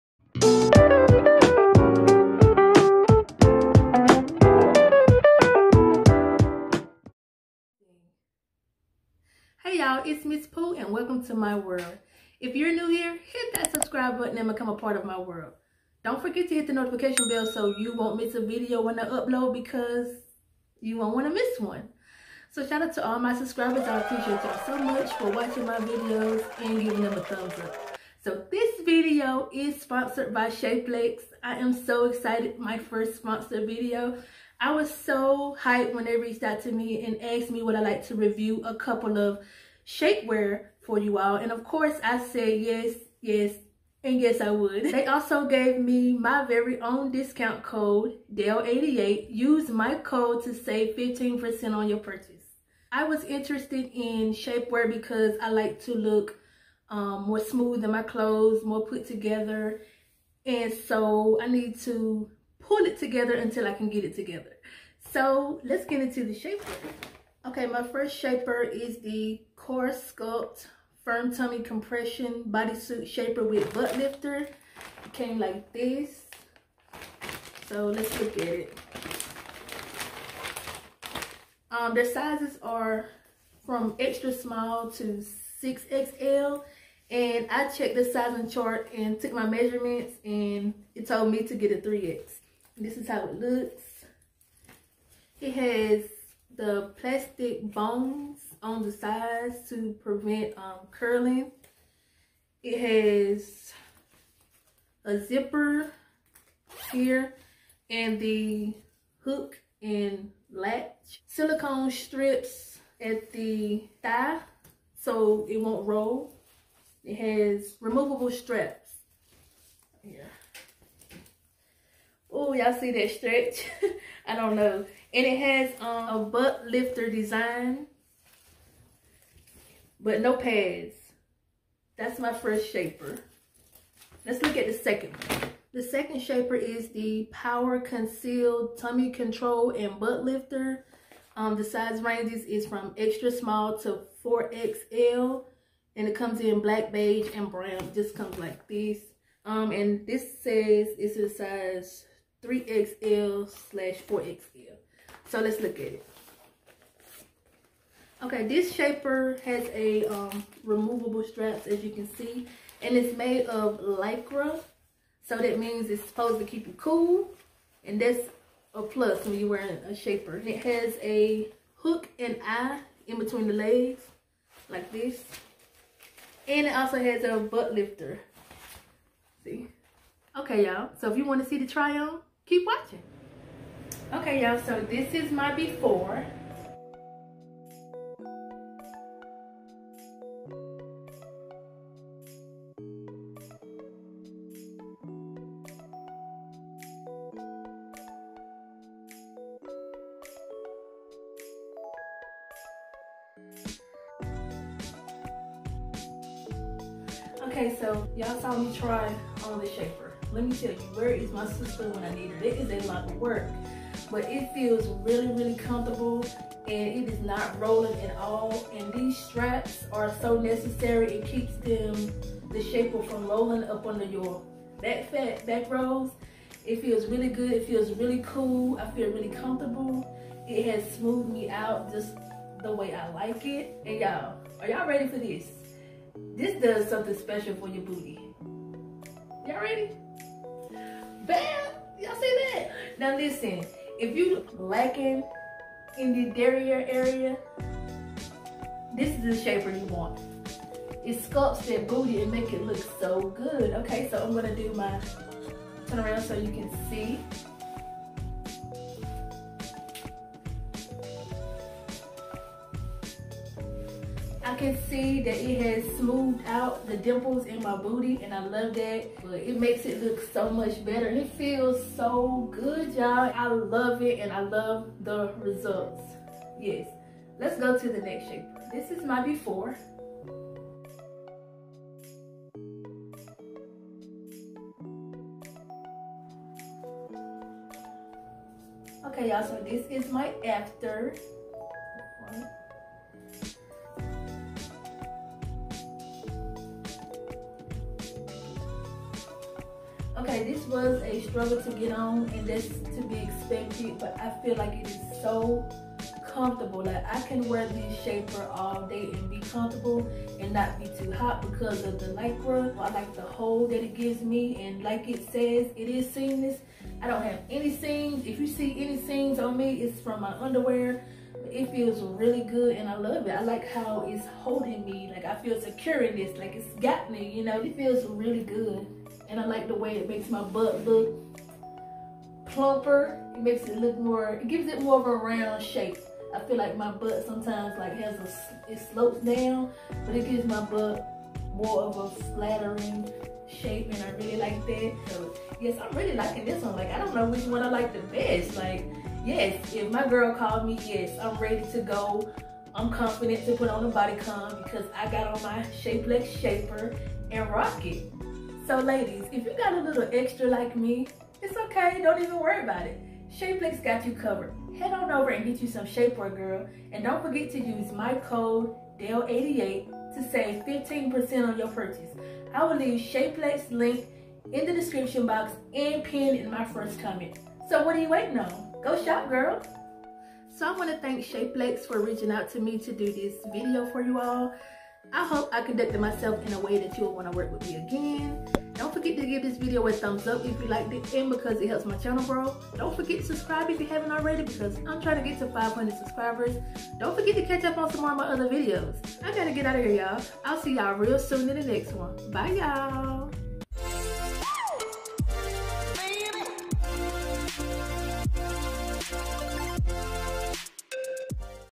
It's Tight But Is It Right?|Shapellx Shapewear Try-On & Review
Also check Shapellx out for your bodysuits, waist trainers and other shapers.

💖SPECIAL THANKS TO SHAPELLX FOR WORKING WITH ME AND BEING SO KIND.

Bear with me, I can't do nothing but get better.

Bye, y'all.
Ms. Pooh💋